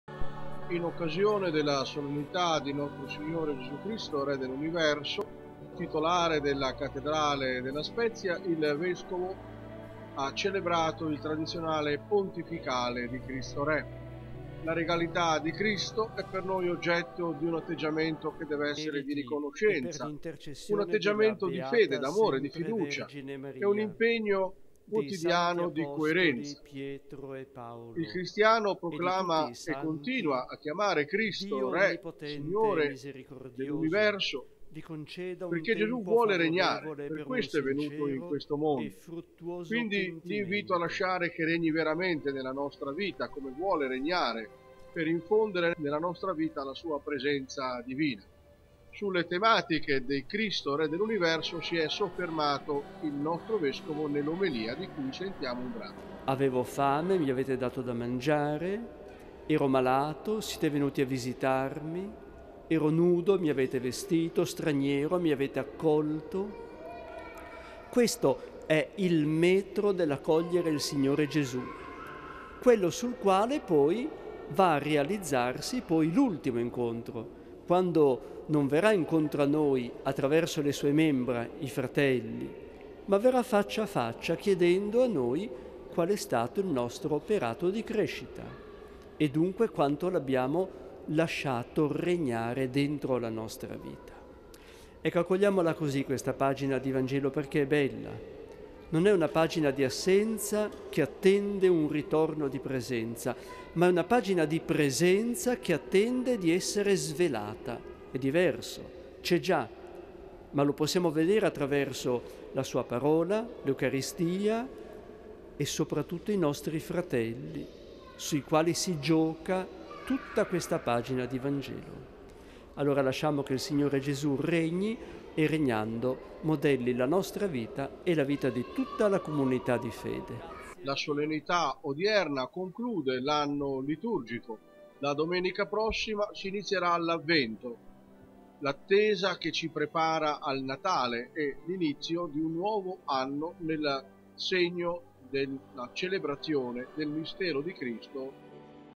Festa di Cristo Re, la messa in Cattedrale alla Spezia 27-11-2017
Ieri la solennità di Cristo Re, con la celebrazione in Cattedrale alla presenza del Vescovo diocesano Mons. Luigi Ernesto Palletti.